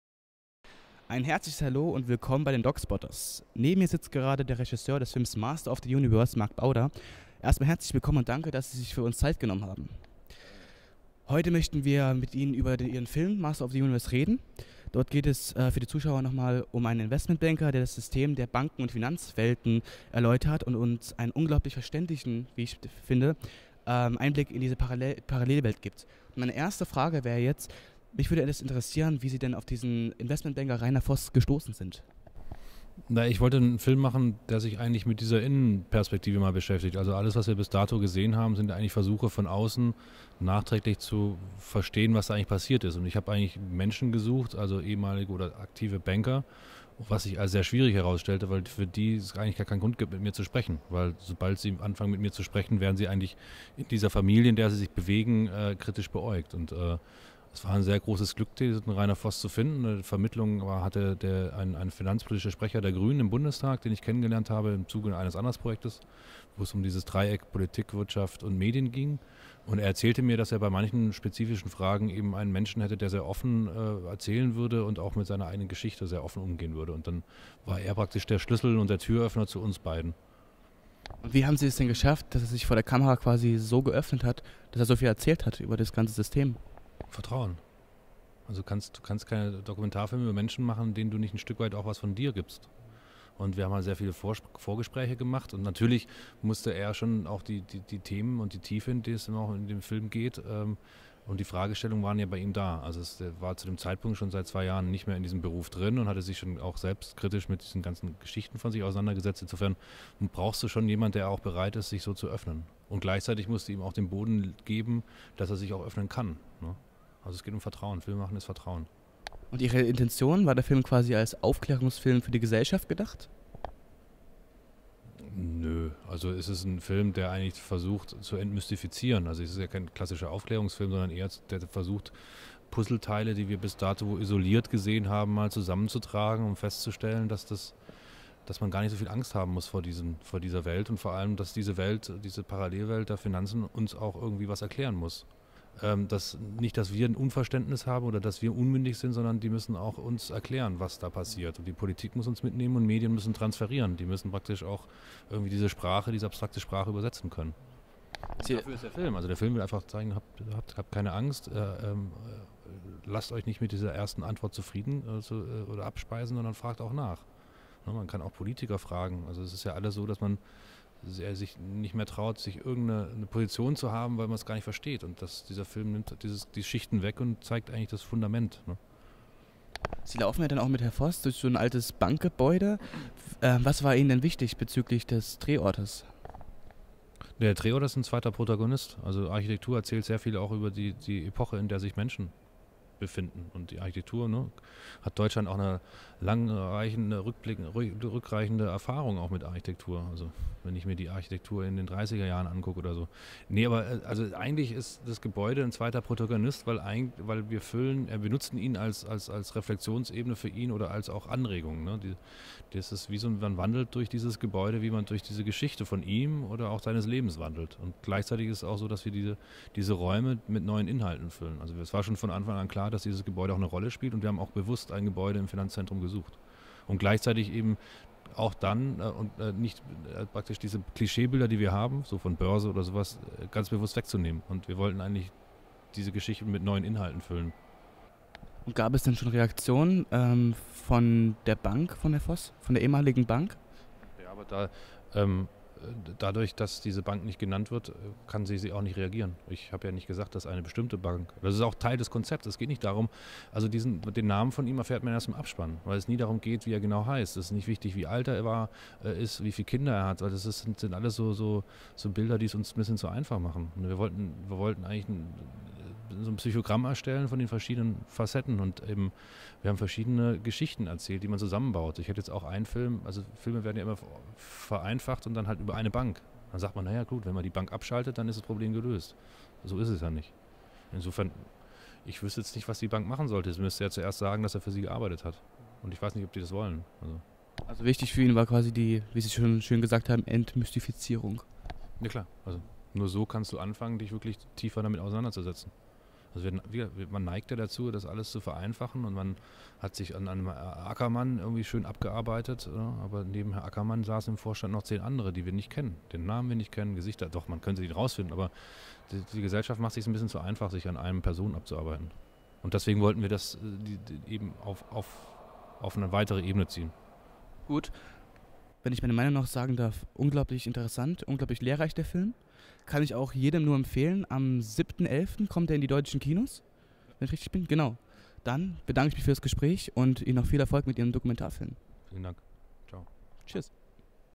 Interview mit Marc Bauder, Regisseur von Master of the Universe
Er war einst ein hochrangiger Investmentbanker. Nun sitzt er in einem verlassenem Bankgebäude in Frankfurt und gibt den Zuschauern einen beängstigenden und seltenen Einblick in die Welt der Banken und Finanzen.

Rainer Voss weiß, was es heißt, „Master of The Universe" zu sein. Der ehemalige Investmentbanker jonglierte damals mit unvorstellbar hohen Geldbeträgen und verdiente dabei selbst noch Millionen. Er kennt die Finanzbranche genau, das System, ihre Macht, ihre Krisen. Denn er war lange ein Teil dieser „Familie". Verständlich und zugleich beunruhigend klärt er uns über das geschlossene System der Banken auf und gibt gegen Ende sogar eine Besorgnis erregende Prognose.

Der Regisseur Marc Bauder verfolgte mit seinem Film „Master of The Universe" ein klares Ziel. Über seine Intention und über die Frage, wie er auf den ehemaligen Investmentbanker Rainer Voss gestoßen ist, haben wir uns mit ihm unterhalten.